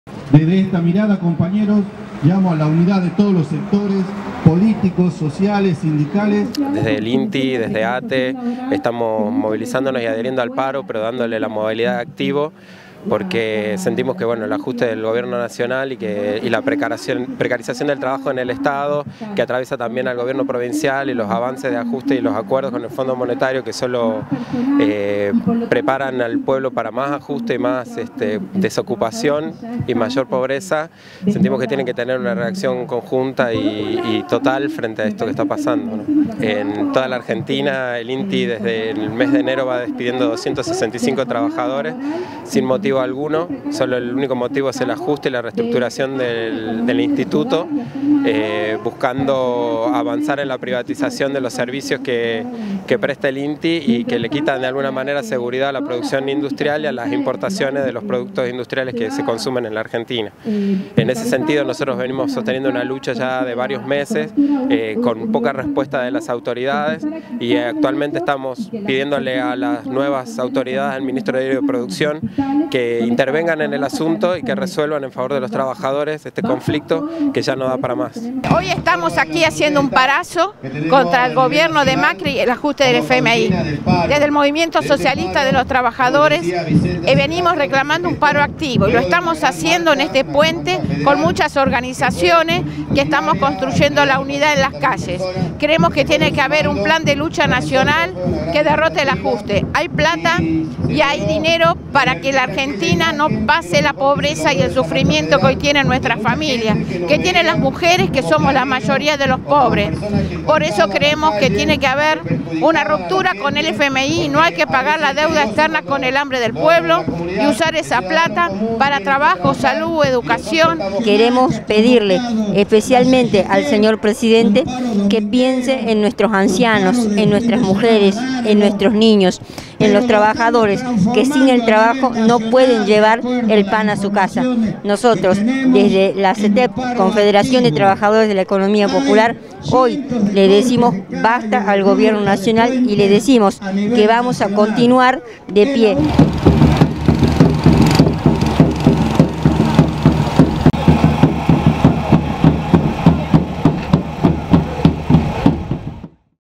Tercer paro contra el gobierno de Macri: cortes y movilizaciones
En reclamo contra el ajuste y el acuerdo con el Fondo Monetario Internacional, el tercer paro nacional contra la administración de Cambiemos se siente en la provincia en al menos una decena de cortes de rutas y puentes.